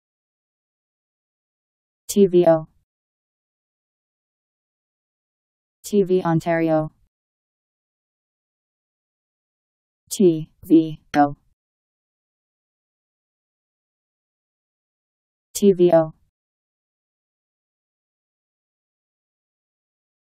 TVO Meaning
Video shows what TVO means. TV Ontario.  TVO pronunciation. How to pronounce, definition by Wiktionary dictionary. TVO meaning. Powered by MaryTTS